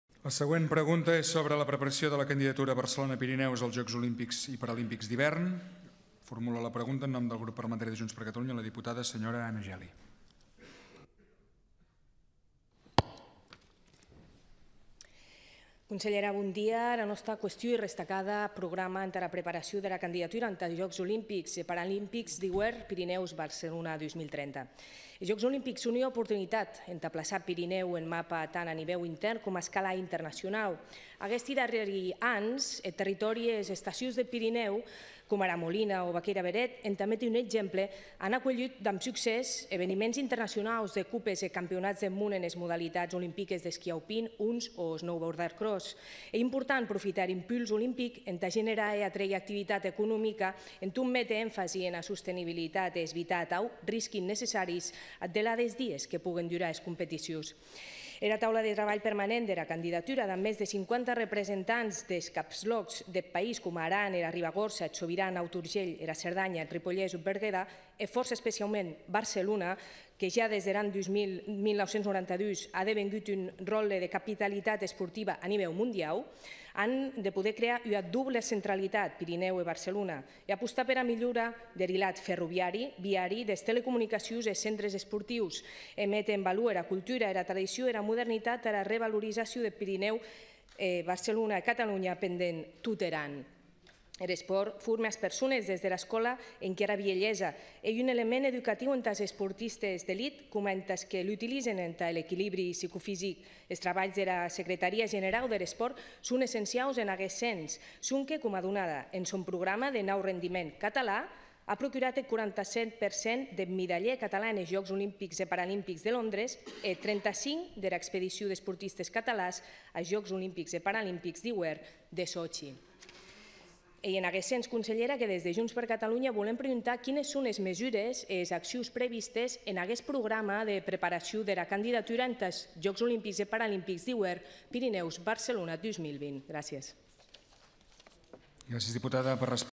Discurs d'Anna Geli - 06.03.2019
La diputada de Junts per Catalunya, Anna Geli, pregunta al Govern sobre la preparació de la candidatura Barcelona-Pirineus als Jocs Olímpics i Paralímpics d'Hivern.